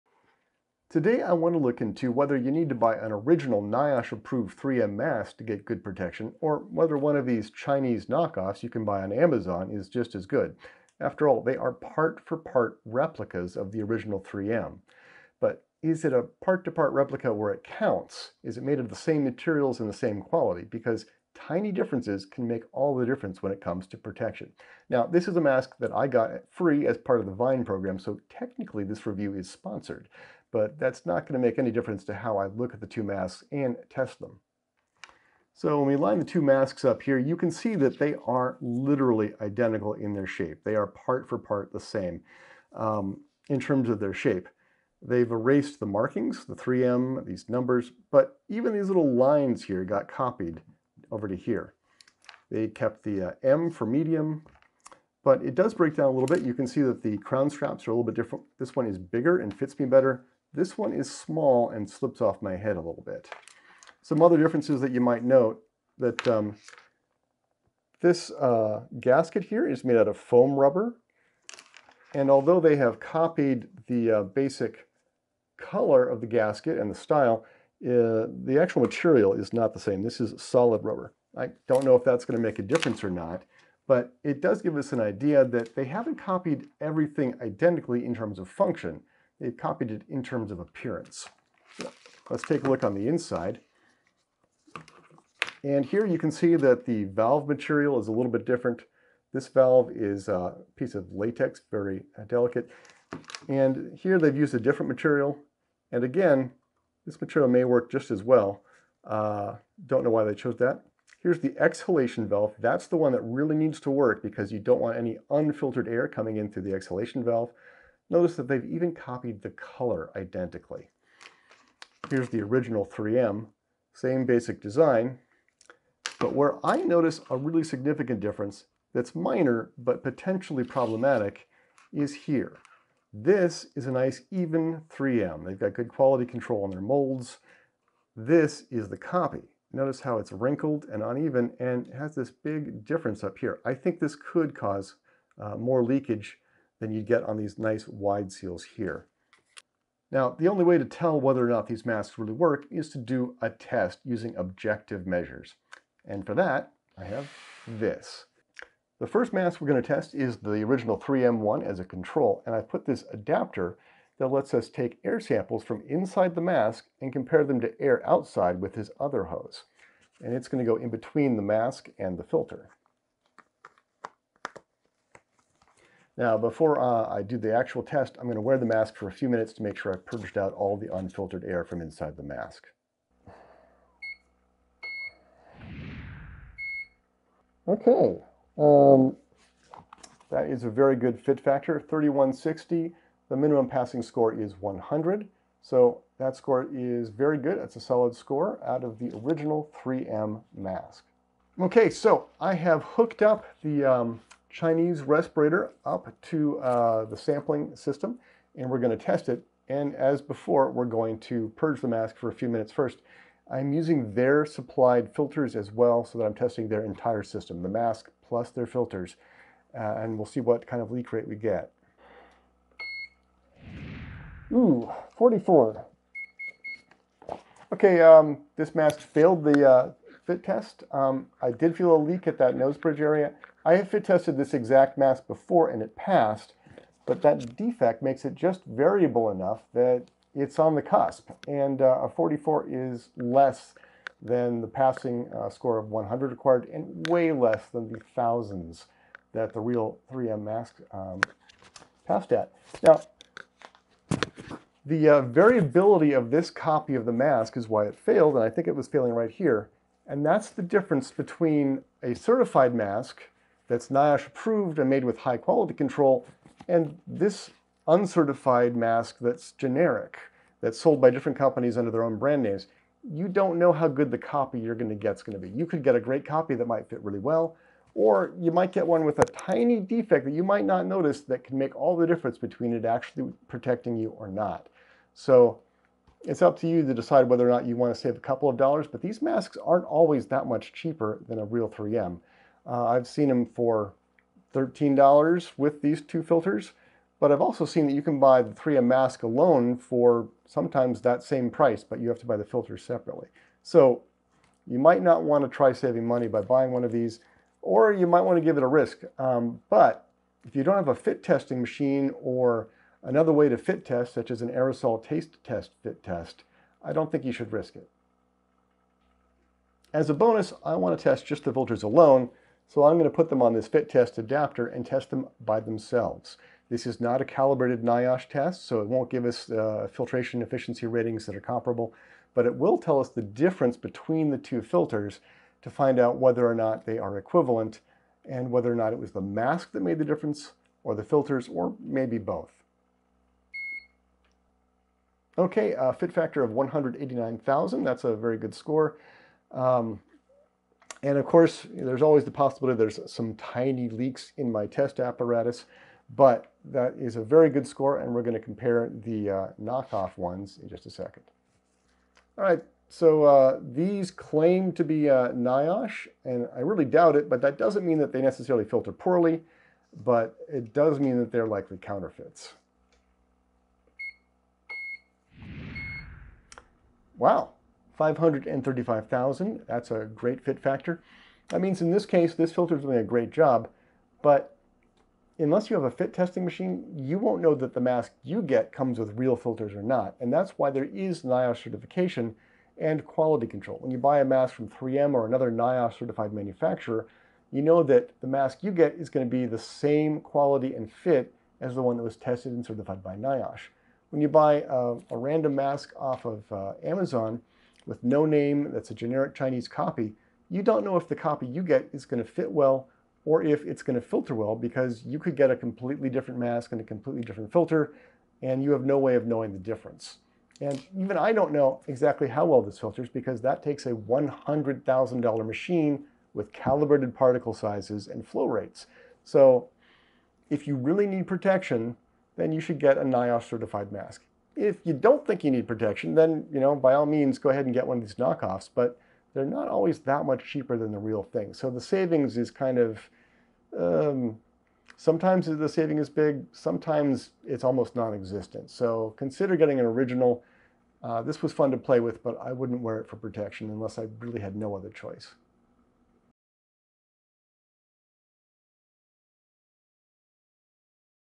Functional or Fake? Mask Fit Test of Generic Copy of 3M Respirator
Update 1/18/2026: NIOSH tested knockoffs of 3M filters, and all but one model failed.

Amazon has now removed most but not all 3M knockoff respirators and filters.

Original Description:
Are the generic copies of 3M 6200 respirators you can buy on Amazon safe? They look just like the 3M originals, but do they actually work? I put one I got free from the Vine program to the test using a mask fit testing machine.

The copies you can buy on Amazon are part for part replicas of the 3M originals - so faithful they appear to be molded directly off of 3M masks. But appearances can be deceptive. It's possible to make a replica that has all the appearance of the original, but is more like a movie prop and doesn't have the functionality. And with respirator-grade masks and filters, tiny details can make a huge difference in performance.

Real 3M respirators are NIOSH approved, and NIOSH has strict requirements and audits for consistent quality control to insure that you get a mask that performs identically to the ones approved by NIOSH. Generic respirators have none of that. You could get a great mask one day and a completely different one that is an utter failure the next. And that is the most important point - this video is only testing one of these masks. If you buy one, the one you get could be better or worse than the one I tested.

In the video I'm using a PortaCount mask fit testing machine that gives scores in "Fit Factor".

Fit factor is the the concentration of ambient particles outside the mask divided by the concentration inside the mask. So if there were 1000 particles outside and 10 inside, 1000 divided by 10 is 100, so the air inside the mask is 100 times cleaner and the fit factor is 100.

The Portacount counts particles from .02 to 1 micron in size.